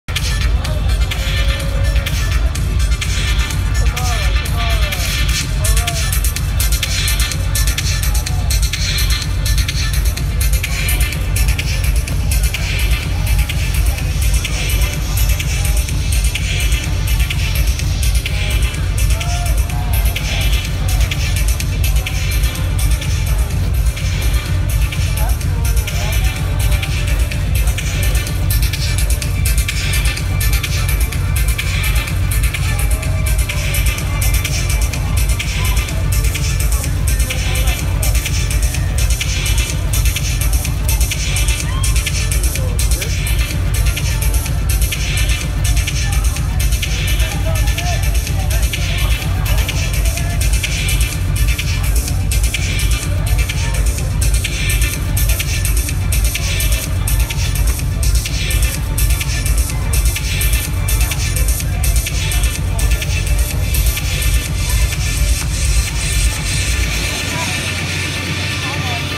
Shuffling | Factory93 | Eric Prydz
Dancing in front of the mural outside of Factory 93 during the Eric Prydz show; Fri April 7.